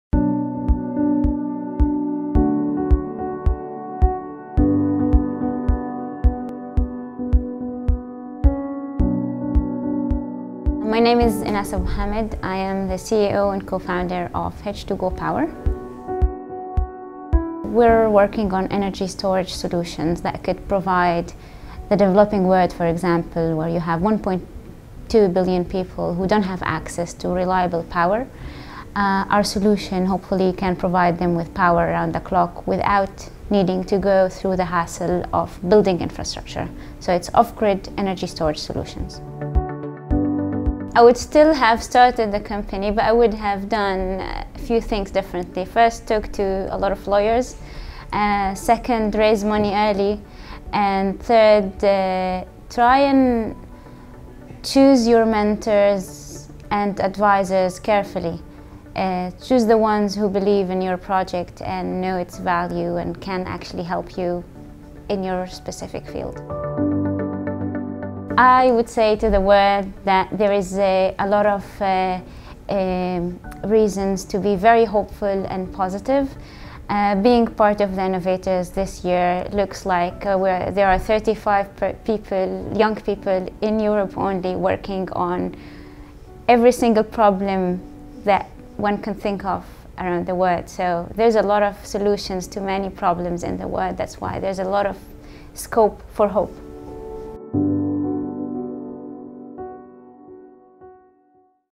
Efficient storage of electricity from renewable sources – Enass Abo Hamed
Meet Enass Abo Hamed, selected among the 35 Innovators Under 35 Europe in 2017. Efficient storage of electricity generated from renewable sources will be possible through her project, which enables hydrogen storage and electricity production on-demand.

Special thanks to Le Bloc Paris for providing a space to manage and record all the interviews with the laureates.